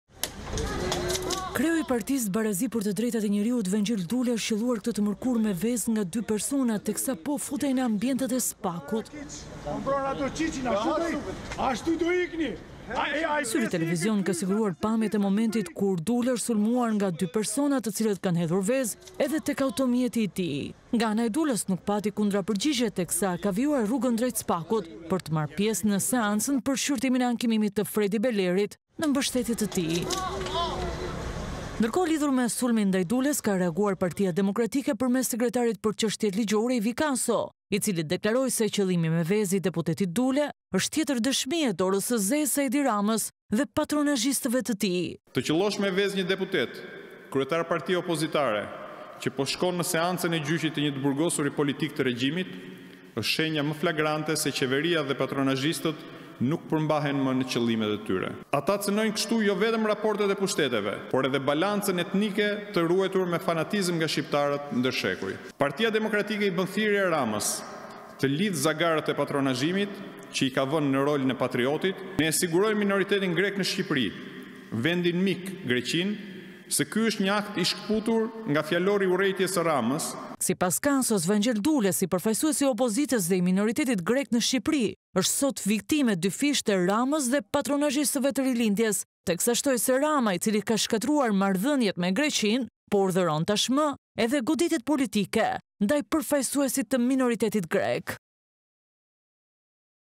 2 persona sulmojnë Vangjel Dulen teksa hynte në SPAK, PD: Sulm politik i urdhëruar nga Edi Rama
2 persona sulmojnë Vangjel Dulen teksa hynte në SPAK, PD: Sulm politik i urdhëruar nga Edi Rama

2 persona qëlluan këtë të mërkurë me vezë kreun e PBDNJ, Vangjel Dule teksa po hynte në ambjentet e spak-ut në mbeshtetje të kryebashkiakut të zgjedhur të himarës,  fredi beleri.   Dule zgjodhi të mos i përgjigjej  sulmit, ndërsa  partia demokratike  akuzoi Edi Ramën si urdhëruesin e goditjes politike ndaj përfaqësuesit të minoritetit grek.  Në një deklaratë për mediat, sekretari për çeshtjet ligjore  Ivi Kaso deklaroi se rasti është dëshmi e dorës së zezë të Ramës dhe patronazhistëve të Rilindjes.